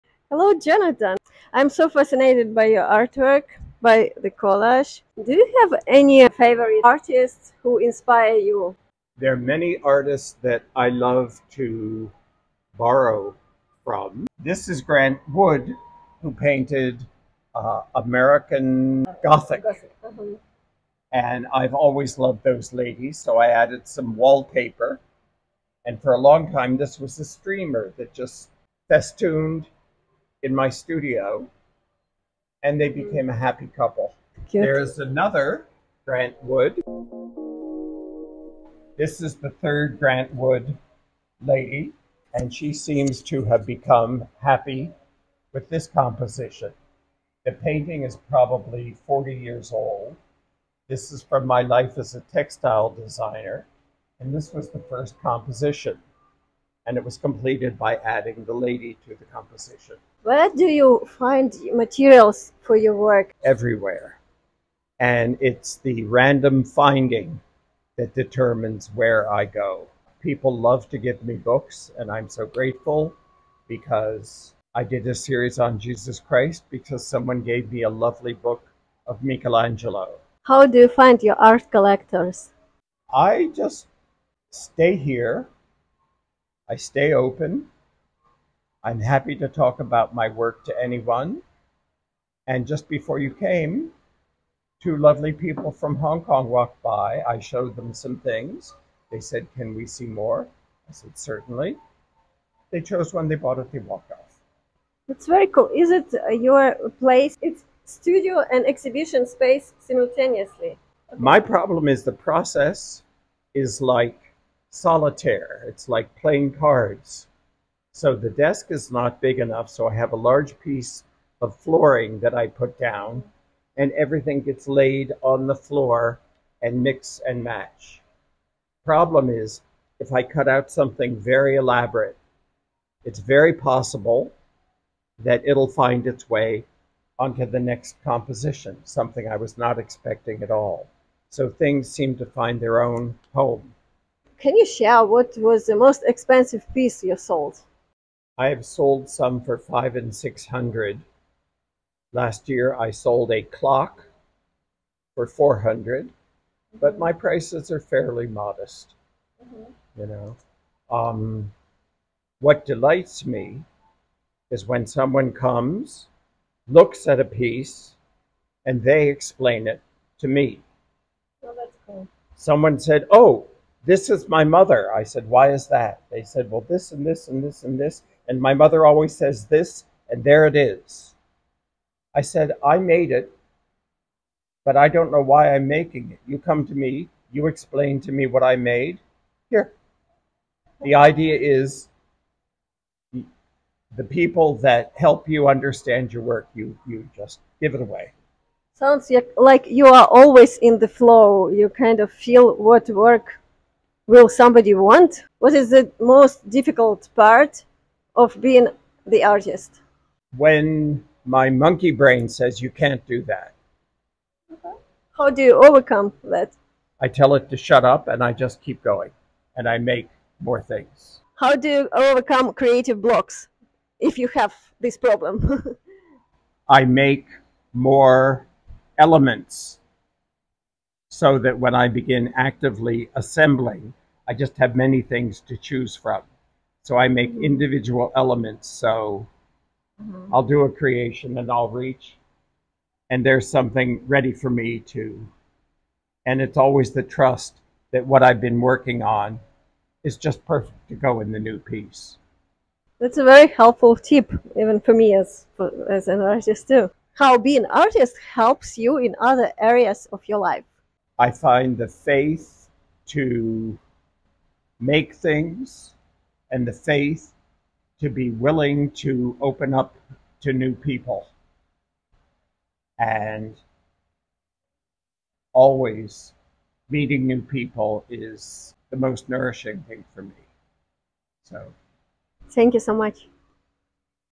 Jonathan Preston Fischer
🎨 Captivating collage creations by Jonathan Preston Fischer, the artist from Brooklyn Waterfront Artists Coalition @bwacredhookart Watch to learn about his intuitive creative process and unique connection with art enthusiasts and collectors🖼️

Visiting an artist's studio is like stepping into a realm of creative enchantment, where colors, patterns, and the artist's passion come to life, filling the air with an infectious sense of excitement✨